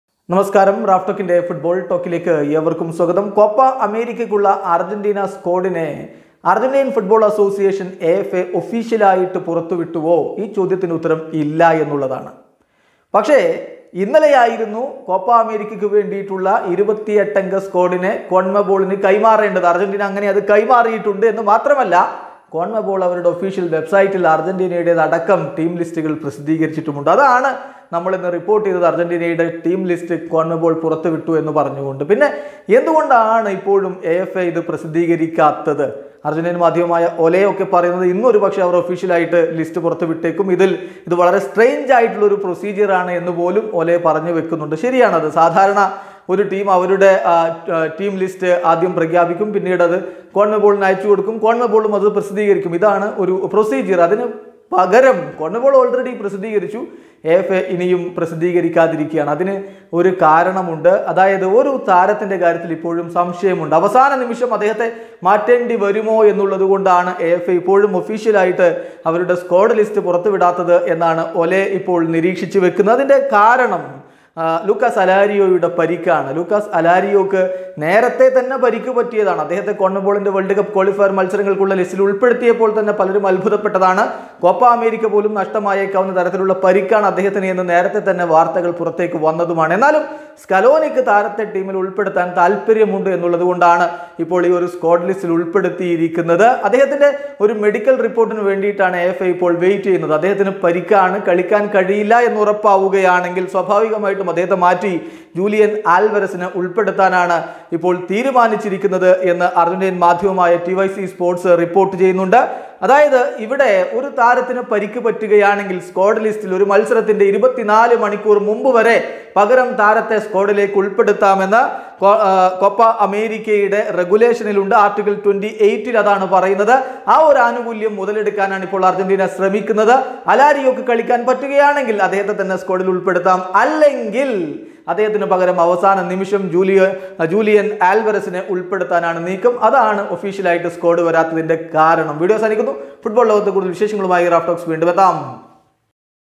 കോപ്പ സ്ക്വോഡ് AFA പ്രഖ്യാപിക്കാത്തതെന്ത്?അർജൻ്റീന ടീമിൽ മാറ്റമുണ്ടാവുമോ? | Football News
Copa America
Argentina football Team